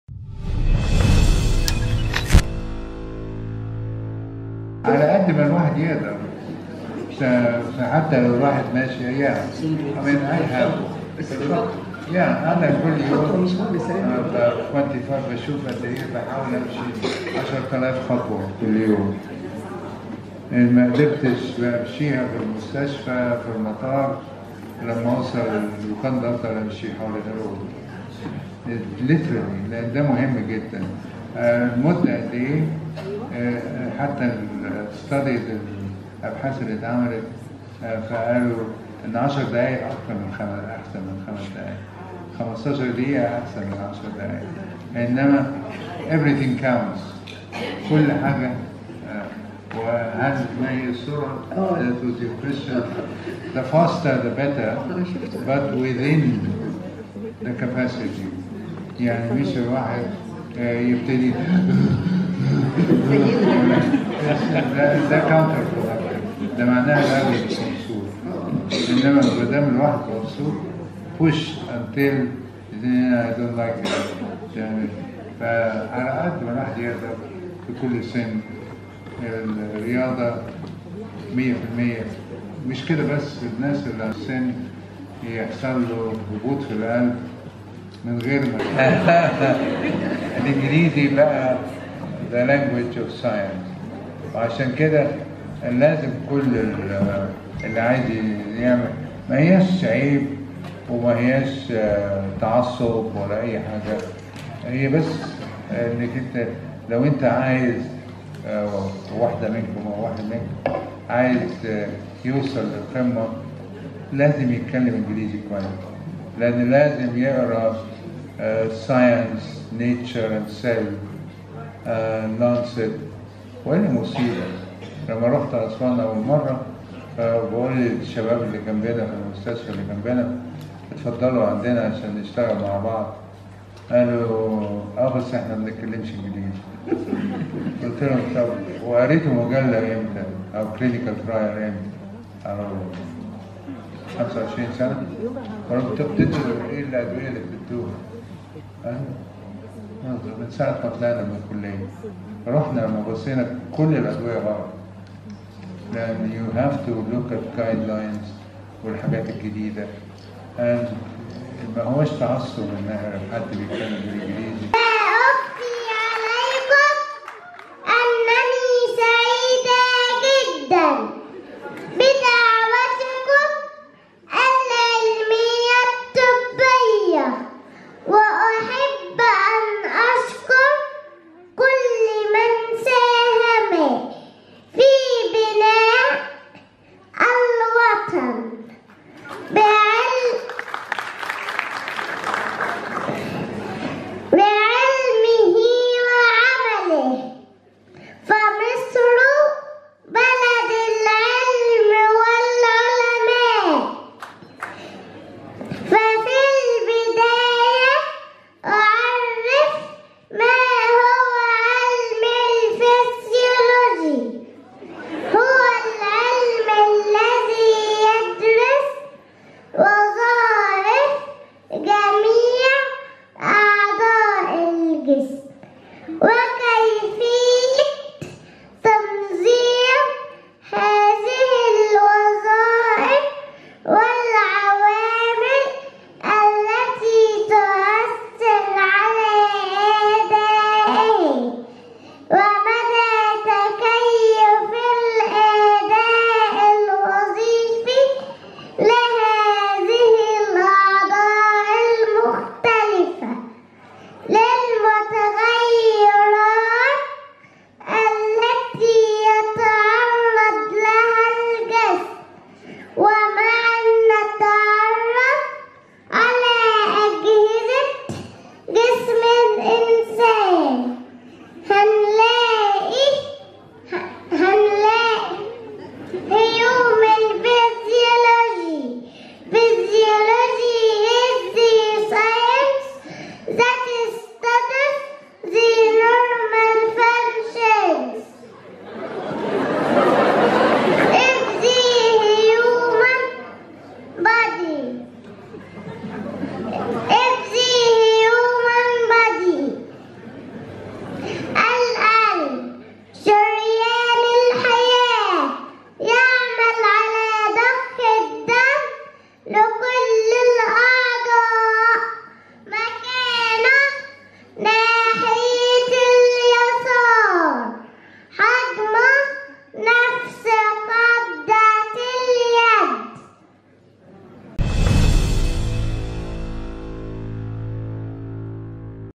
تعرف على نصائح مجدى يعقوم لقلب قوى وحكاية طفلة أبهرته
فى احد الندوات التى القاها العالم وجراح القلب العالمي السير مجدى يعقوب، بوصف الطريقة السحرية لتقوية القلب ووقايته من الأمراض ،فقال خلال المحاضرة التى ألقاها بالمركز القومي للبحوث اليوم  الأربعاء،